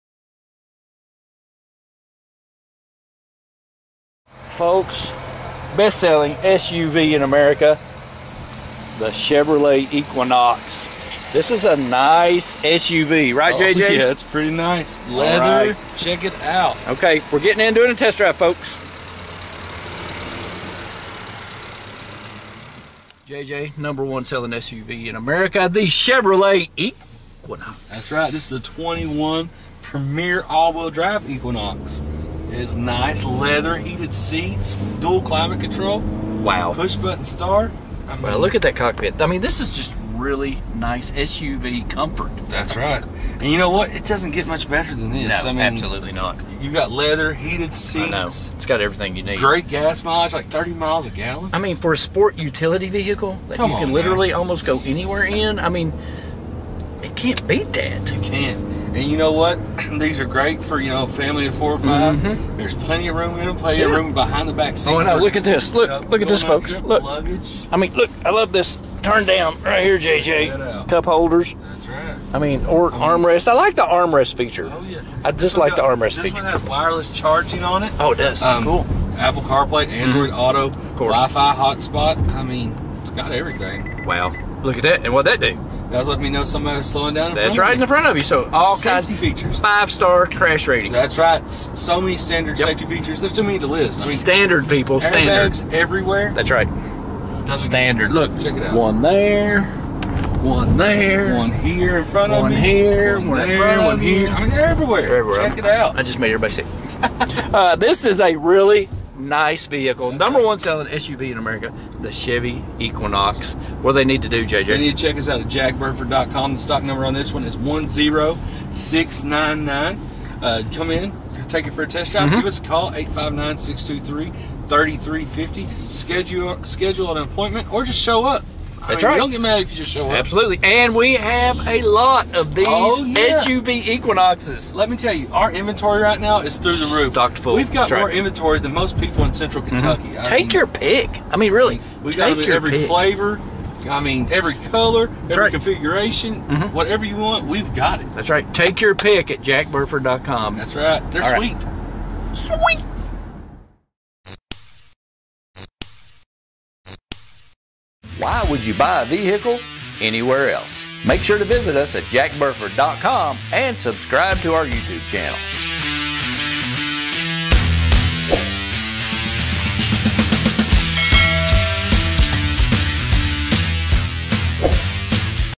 Best Selling SUV Test Drive
Test Drive in the new 2021 Chevy Equinox Premier!